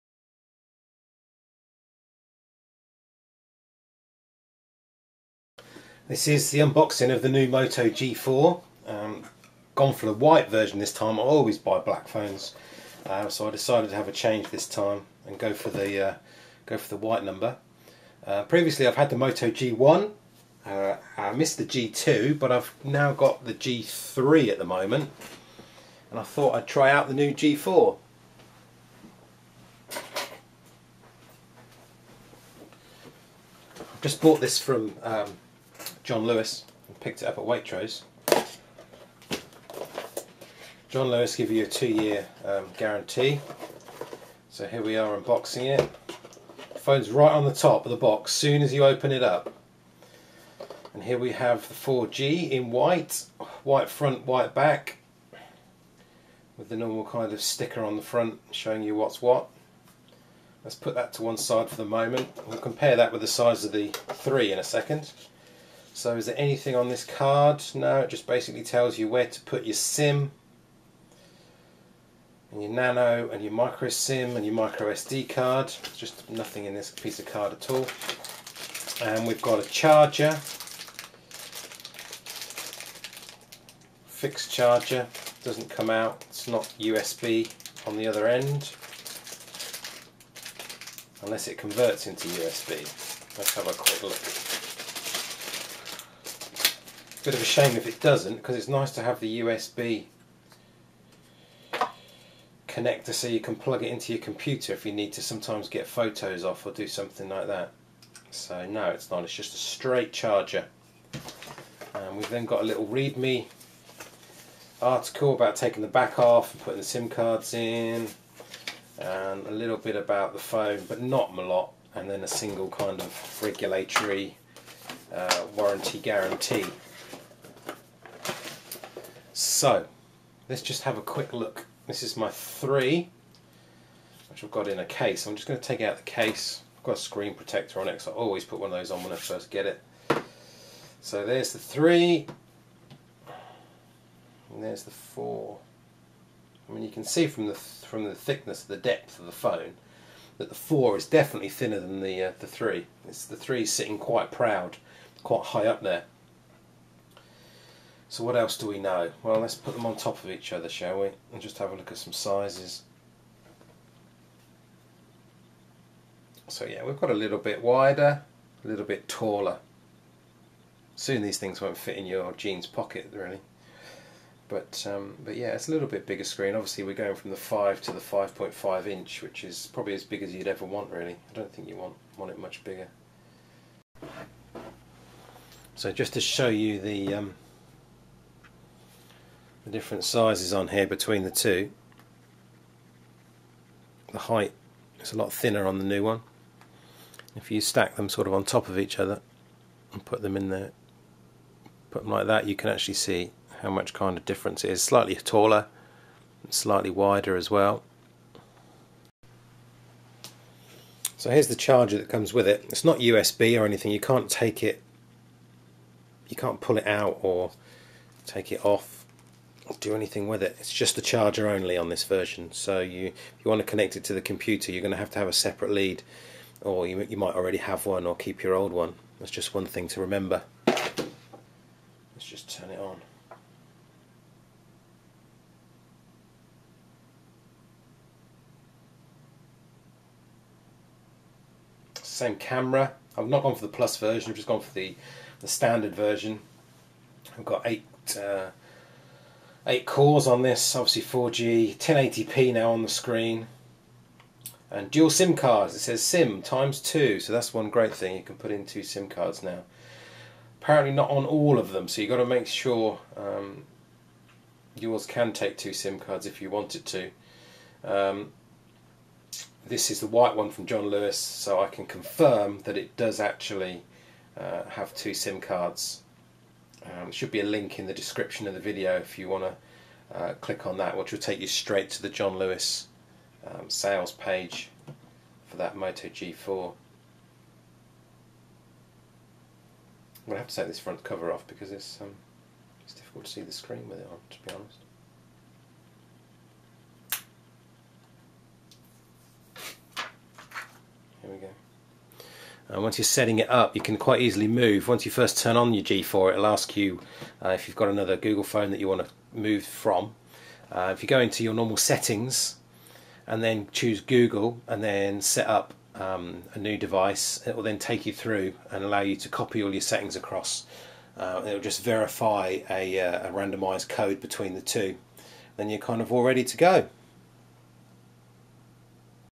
Motorola G4 4th Gen Mobile Phone Unboxing (Moto G4)
Unboxing the new Motorola G4 4th generation mobile phone. I previously owned the 1st and 3rd Gen mobiles. This is the white G4 version with dual sim cards, bought in the UK.

This was purchased in the UK from John Lewis view it